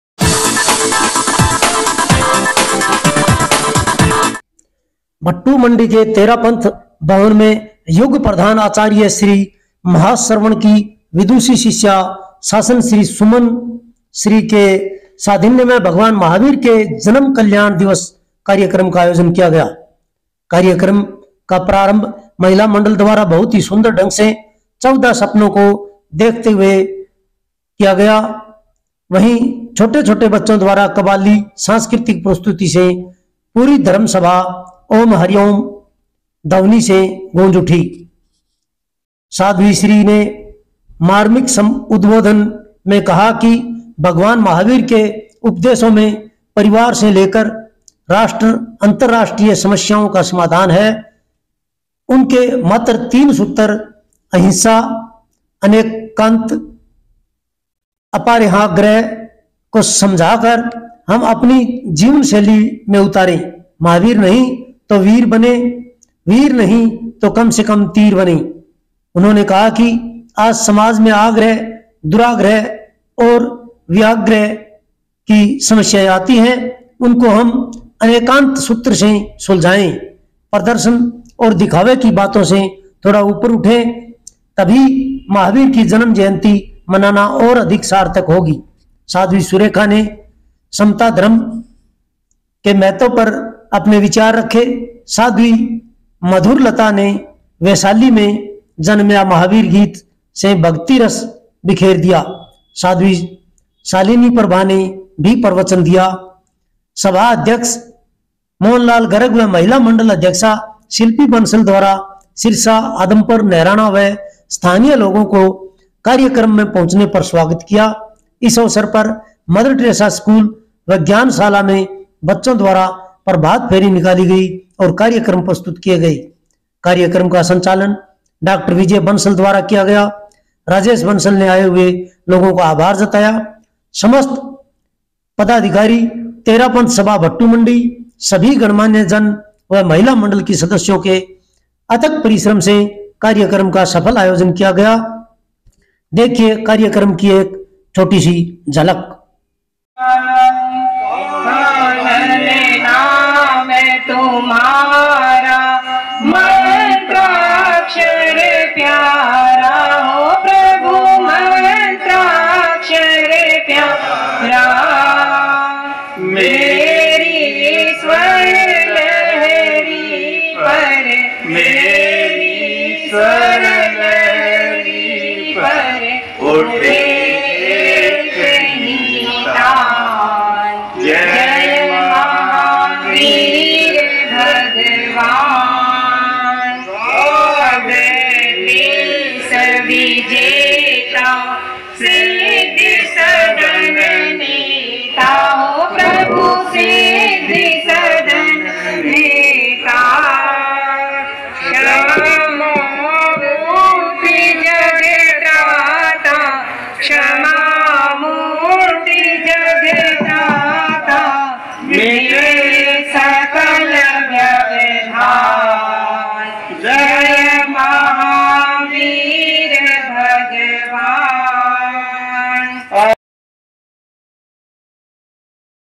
भगवान महावीर जयंती मनाई। तेरापंथ भवन भट्टूमंडी में जैन धर्म अनुयायियों ने मनाया शानदार कार्यक्रम।ओम्
भगवान महावीर जयंती मनाई। तेरापंथ भवन भट्टूमंडी में जैन धर्म अनुयायियों ने मनाया शानदार कार्यक्रम।